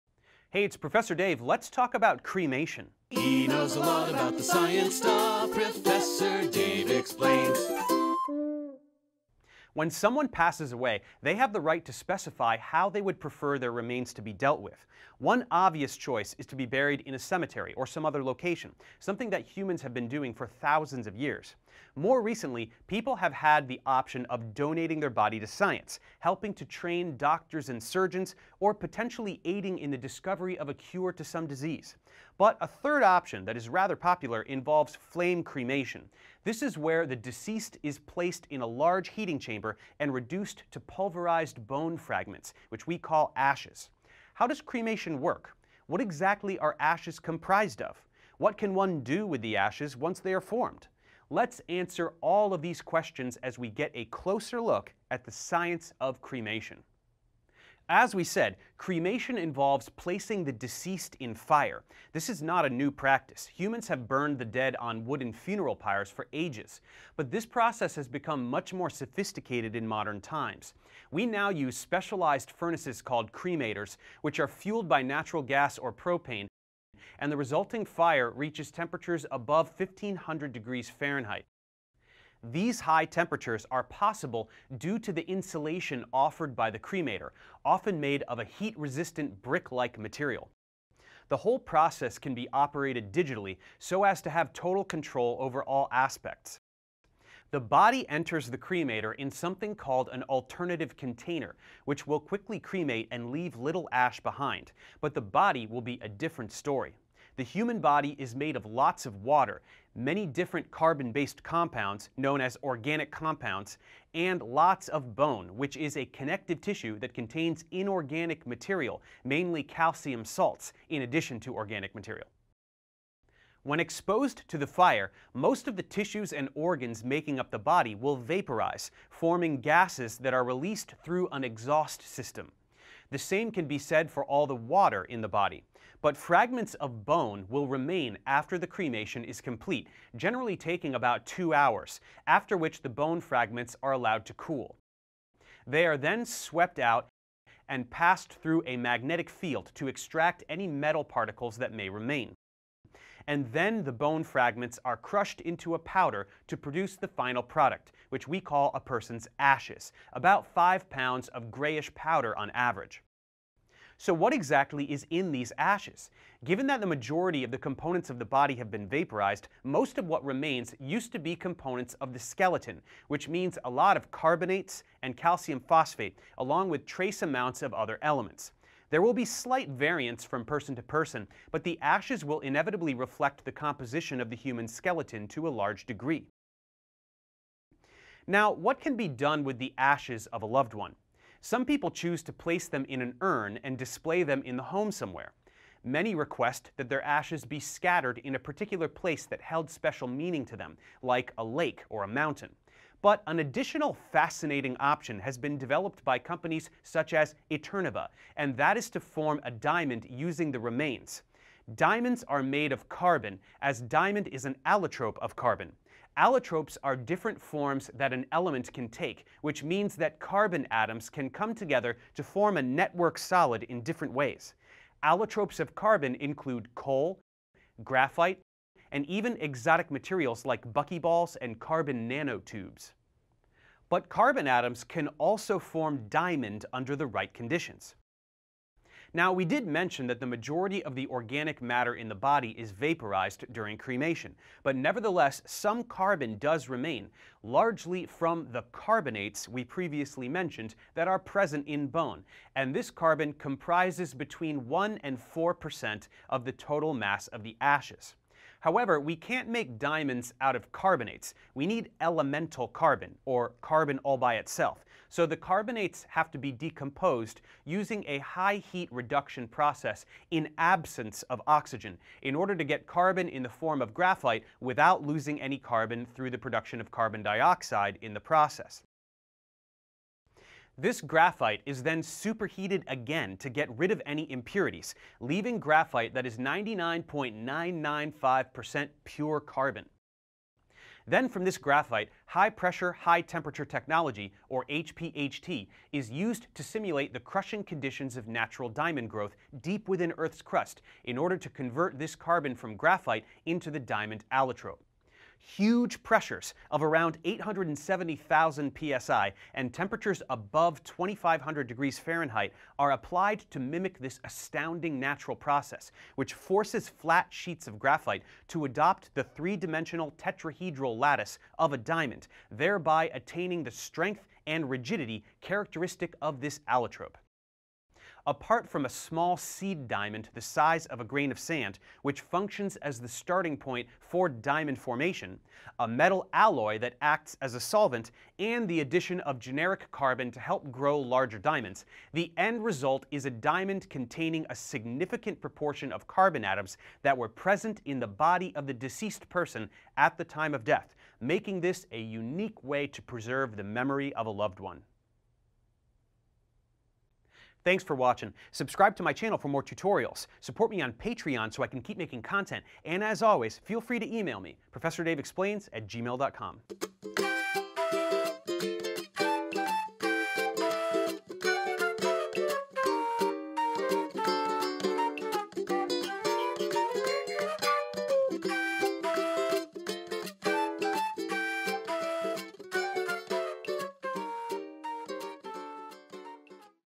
The Science of Cremation and Memorial Diamond Formation
We all have the right to decide what happens to our bodies when we die, and cremation is becoming a popular option. But what is this exactly? How does it work, and what can one do with the ashes? Also, what are diamonds, and how can we manufacture them? Most importantly, what the heck do these two things have to do which each other? Check out this video and find out!